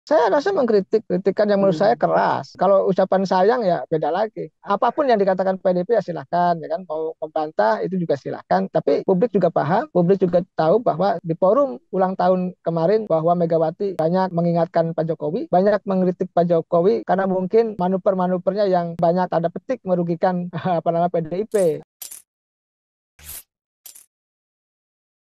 MEGAWATI DISEBUT BANYAK KRITIK JOKOWI DI HUT KE-50 PDIP
Direktur Eksekutif Indonesia Political Review (IPR) Ujang Komaruddin berpendapat bahwa di acara HUT PDIP kemarin, Megawati banyak memberikan kritikan yang keras kepada Presiden Jokowi. Megawati menyampaikan kritik secara tak langsung itu mungkin saja karena banyak manuver-manuver Jokowi yang merugikan PDIP.